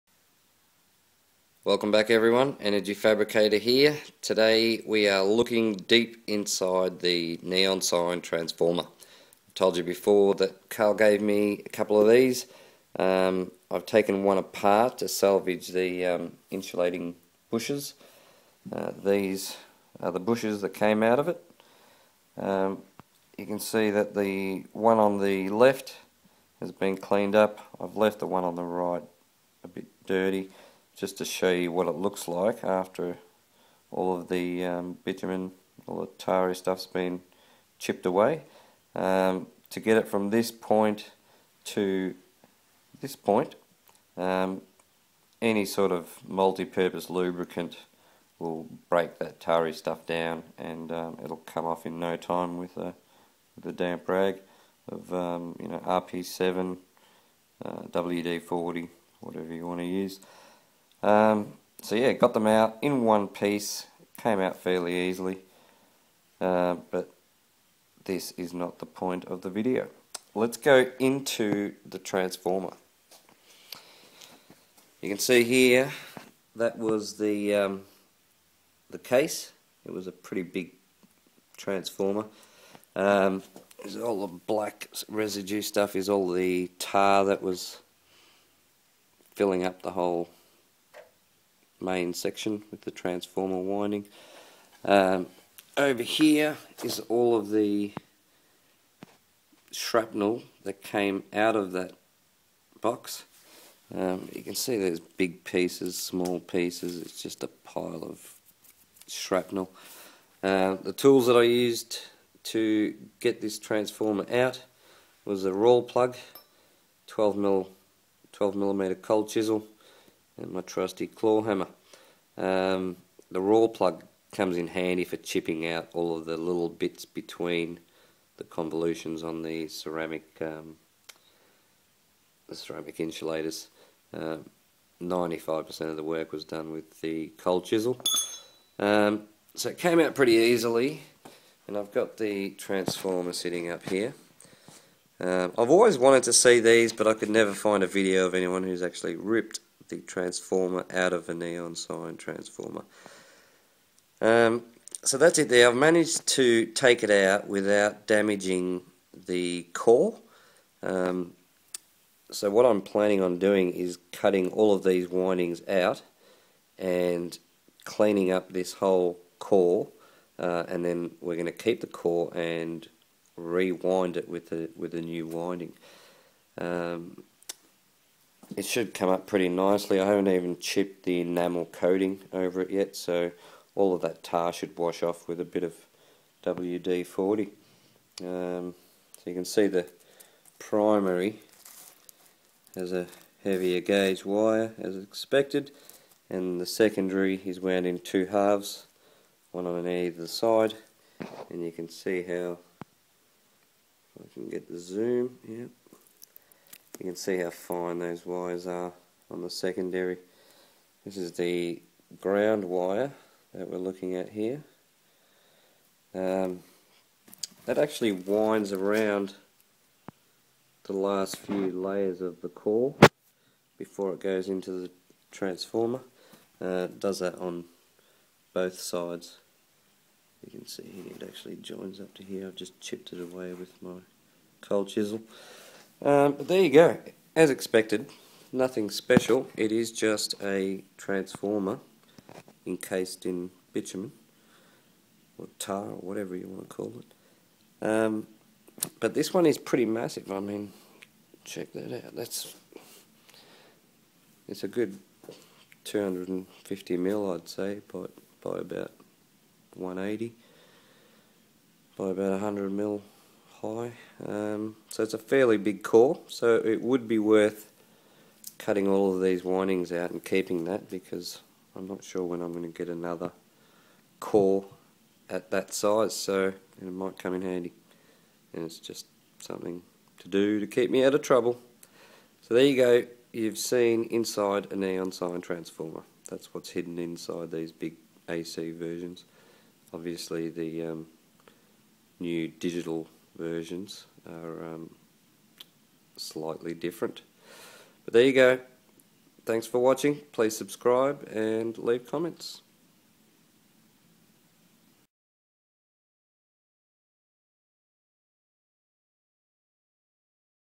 Getting To The Core Of This 11KV, 80mA Neon Sign Transformer (NST) - Insulators, Windings And Core!
I have always been curious to see beneath all the hard bitumen which encases the NST. This is the aftermath of a careful tear-down of a neon sign transformer. I took this one apart to salvage the high voltage ceramic insulating bushes, but then I just kept going to expose the core. No surprises here, just a big transformer buried in Tar.